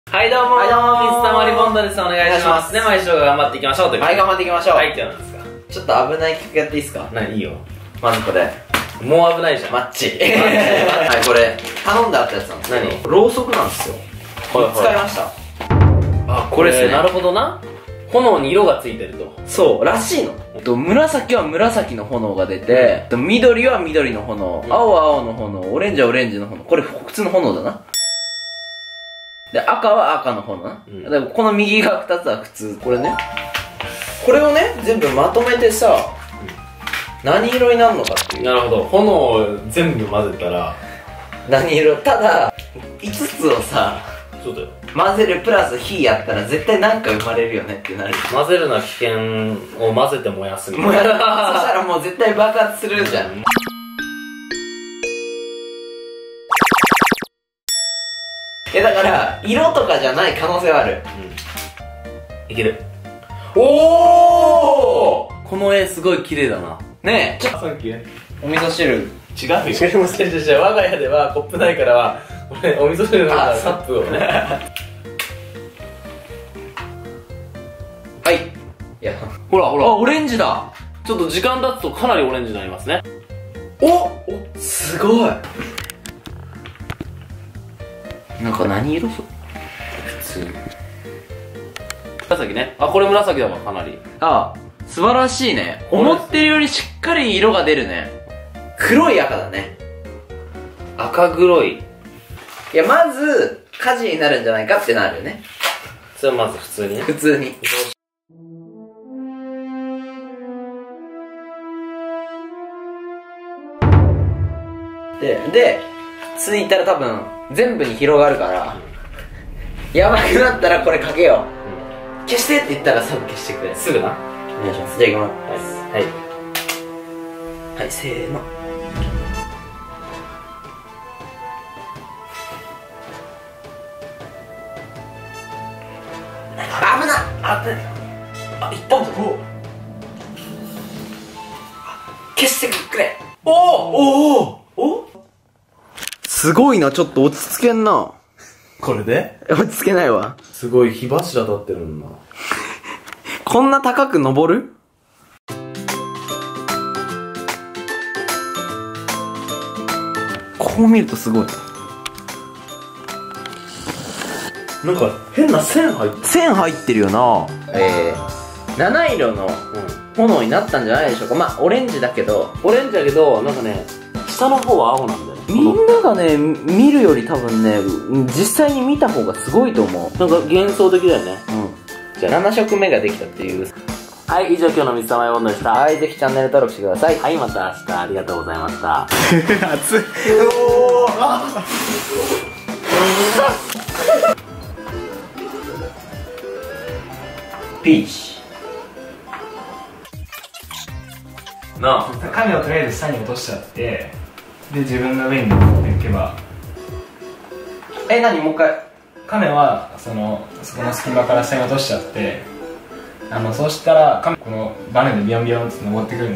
七色の炎を混ぜて虹色の炎を作ったら超危険だった
七色の炎が出るロウソクを混ぜ合わせて虹色の炎作りました！

おすすめ動画↓霊の声が聞こえるドッキリ

一度も欠かさずに毎日２０時に動画配信中！
必死こいて頑張ります。4月は20時ぴったり投稿やります。